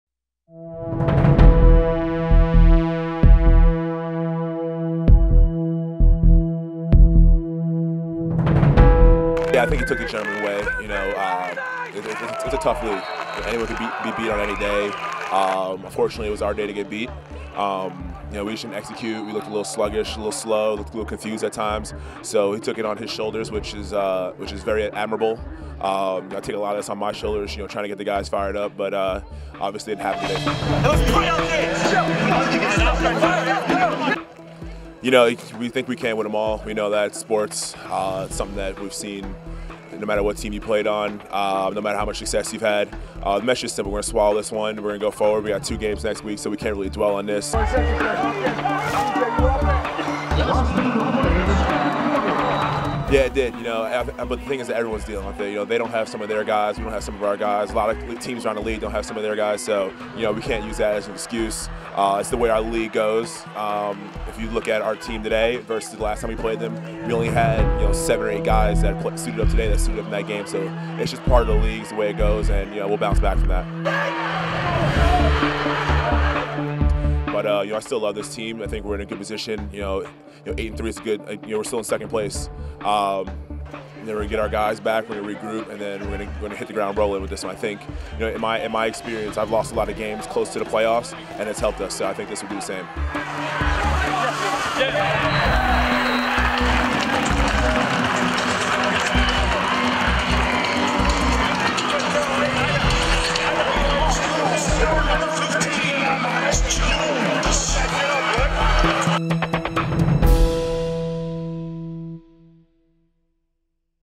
RECAP /// Myles Jones on Loss to Charlotte Hounds
Myles Jones and the Bayhawks are looking to rebound and move forward in their final three games of the regular season after suffering a loss to the Charlotte Hounds on Saturday, July 21st. Watch Myles Jones discuss the game and what the hopes are for the team moving forward.